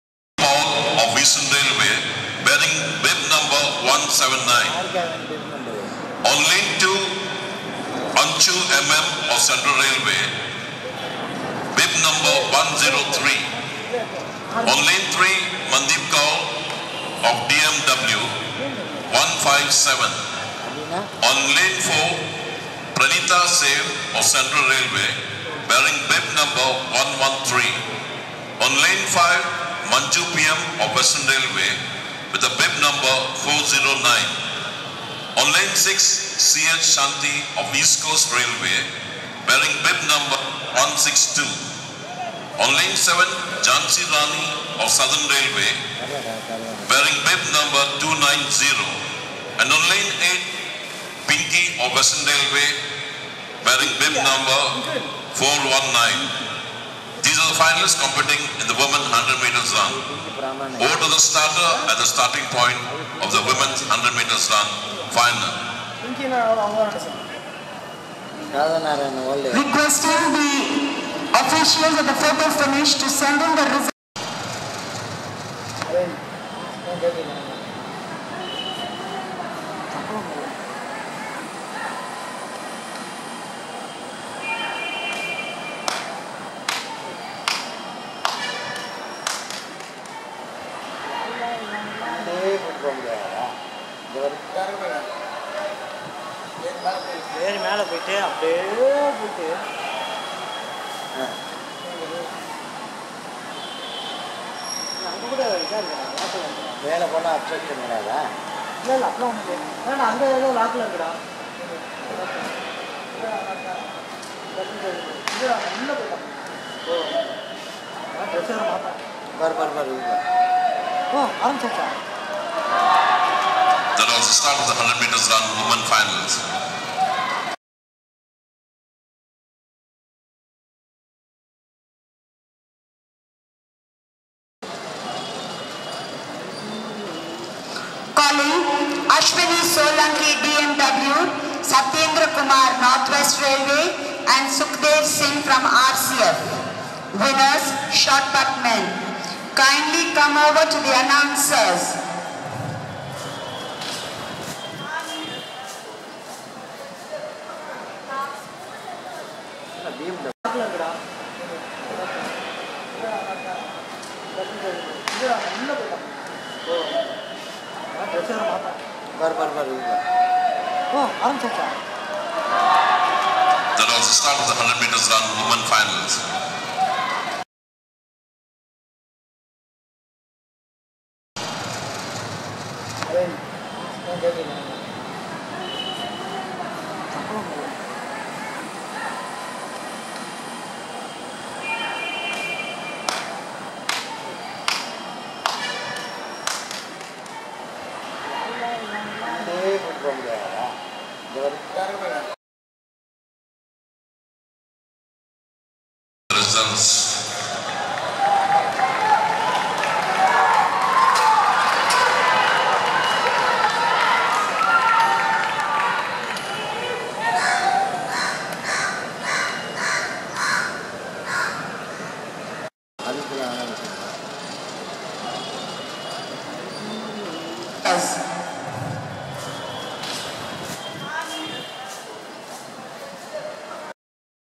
Anchu MM wins 100m. All India 80th Inter Railway Athletics Championships-2014. Chennai
1. Anchu MM          Central Railway      12.23 Sec.
2. Mandeep Kaur    DMW Patiyala        12.39 Sec
3. Pinki                    Western Railway    12.49 Sec
Women's 100 M  Run
Any success for Athletics depends on two factors.
The Athletics will attract viewers and thereby attract  sponsors  and  media coverage.